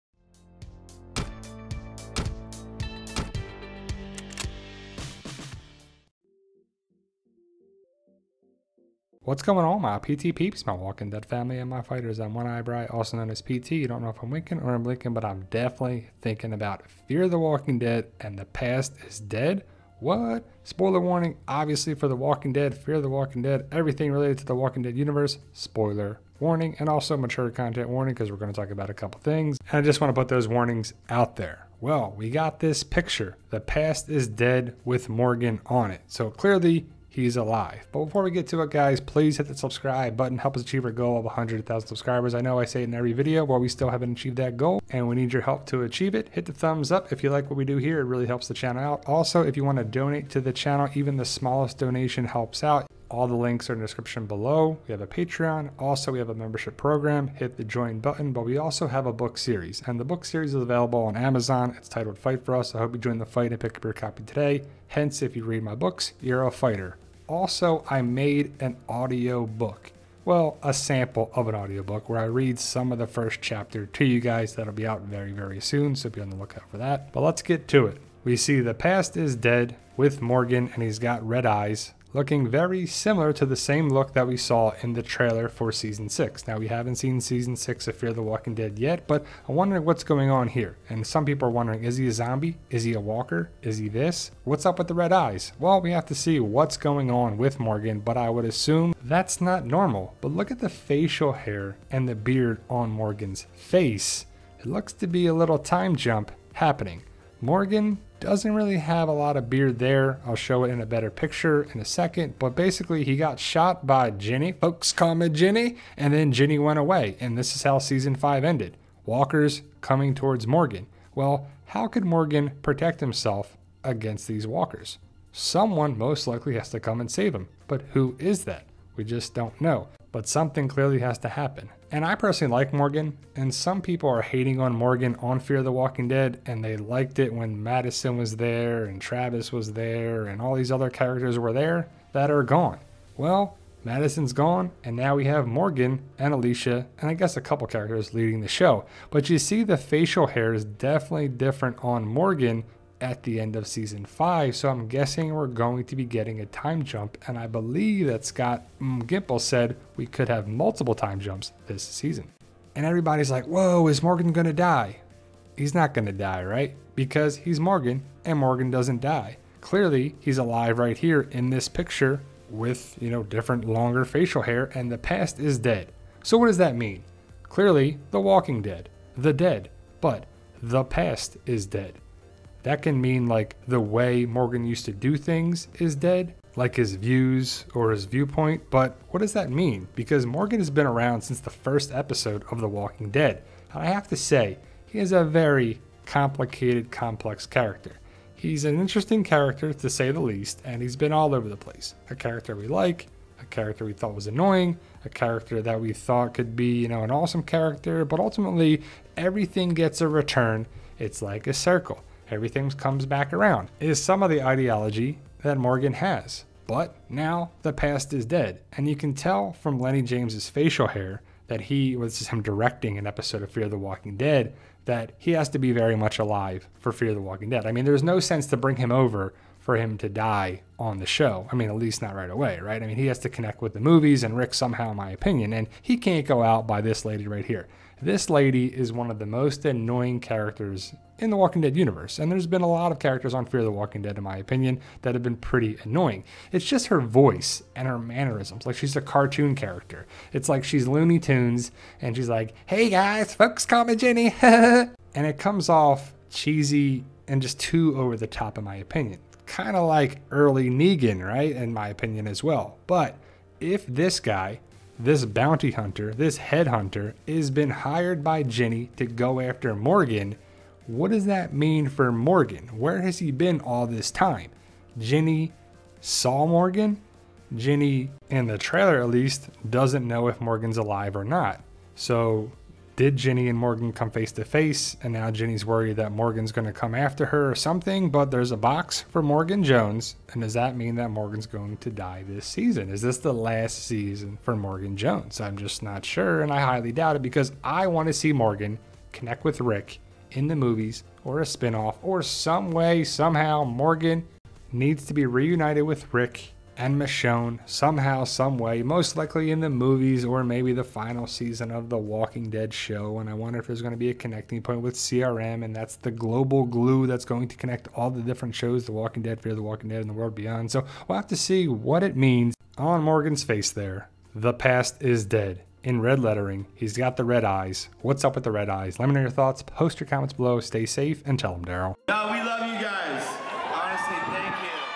Fear The Walking Dead Season 6 Morgan Photo Discussion - What Does It Mean For Morgan?
Spoiler warning for Fear TWD & TWD shows & comic. We discuss, share, highlight, analyze, and theorize a couple things for Fear TWD.

FIGHT 4 US BOOK ORDER:
THE BEGINNING, REUNITED, REFUGE, CONQUERED, AGREEMENT, KEEPER, WEAPONIZE, LEGACY, SETTLEMENT, GHOSTS, ACCESS, POWER